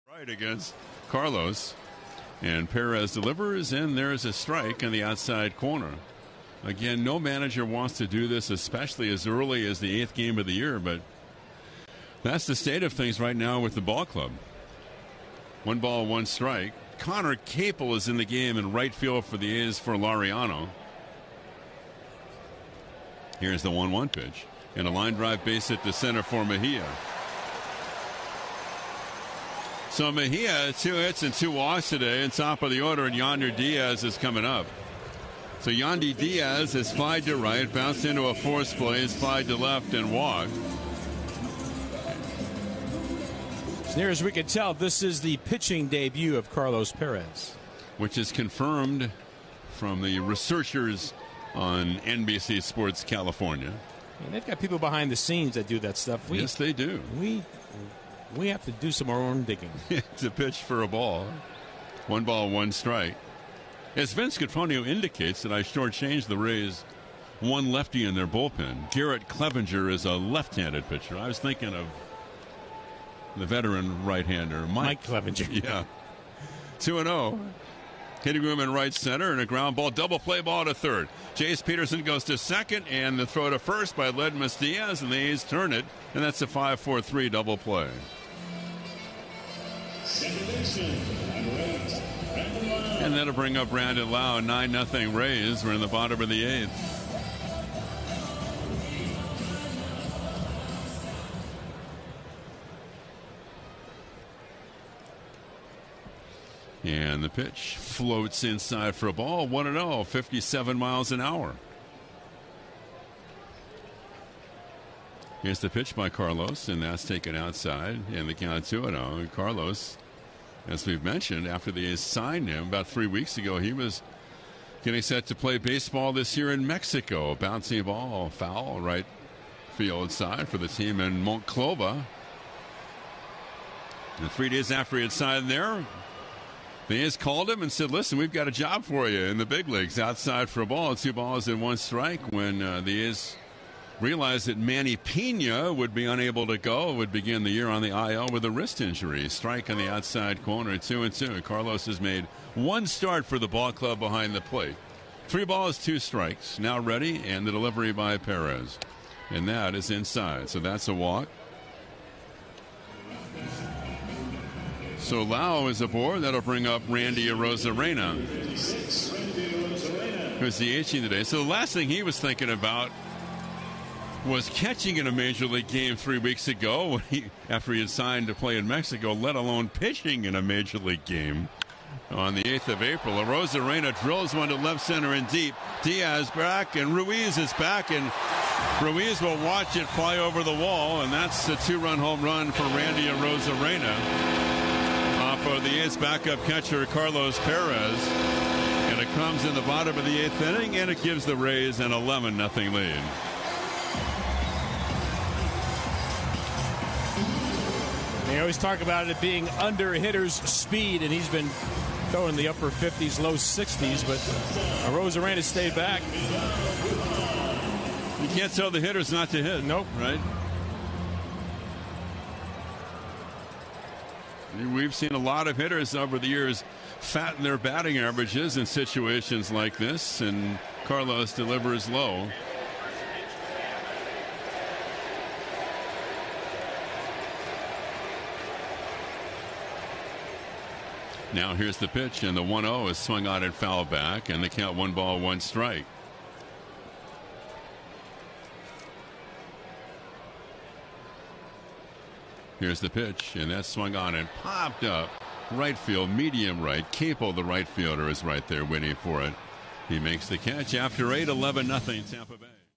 A's backup catcher Carlos Pérez makes MLB pitching debut | 4/8/23
A's backup catcher Carlos Pérez made his MLB pitching debut against the Rays on 4/8/23 as the A's lost 11-0. Perez's line: 1 IP, 2 H, 2 ER, 1 BB, 0 K